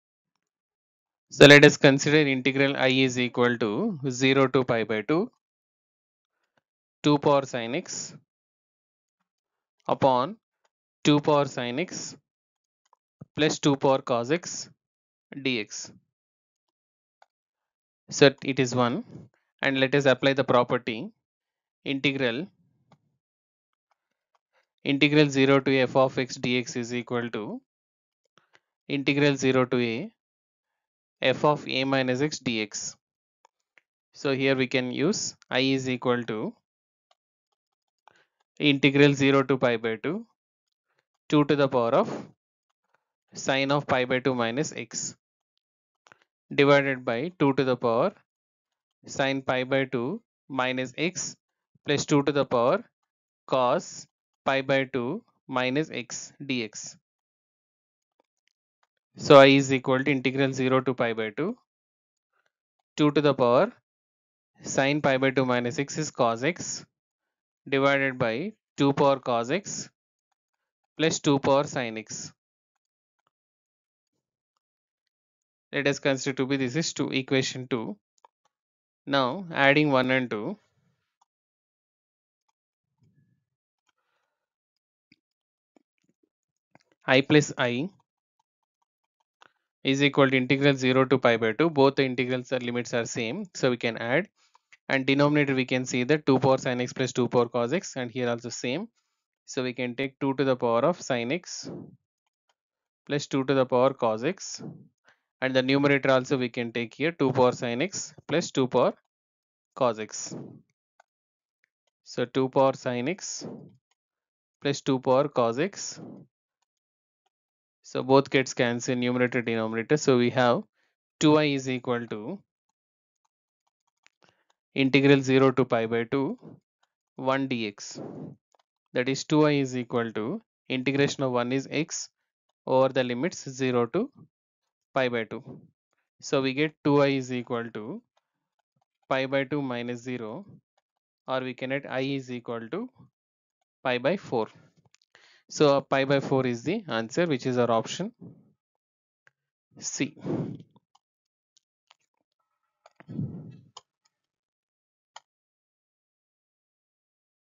Integral 0 to pi/2 (2^sin x/ 2 sin x+2^ cos x) dx equals BITSAT Integrations|definite|VIT|JEE|PYQs
Integrations@FountainofMathematics